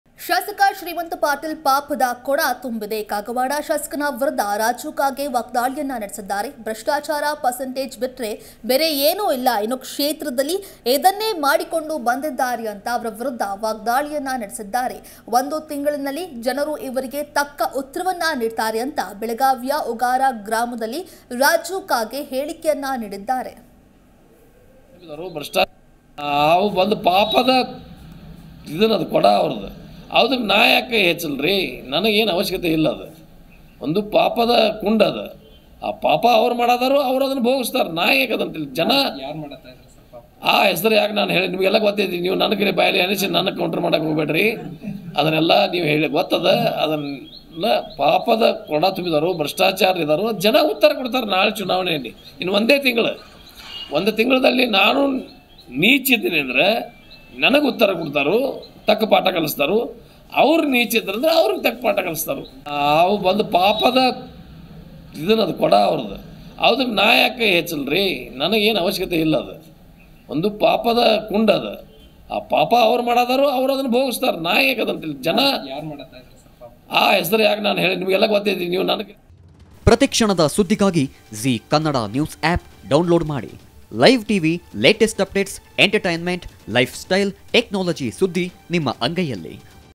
Raju Kage || ಪರ್ಸೆಂಟೇಜ್ ಲೆಕ್ಕ ಬಿಟ್ಟರೆ ಅಭಿವೃದ್ಧಿ ಮರೀಚಿಕೆ;ರಾಜು ಕಾಗೆ || ZEE KANNADA NEWS
- ಶ್ರೀಮಂತ ಪಾಟೀಲ್‌ ವಿರುದ್ಧ ಮಾಜಿ ಶಾಸಕ ಕಾಗೆ ಕೆಂಡ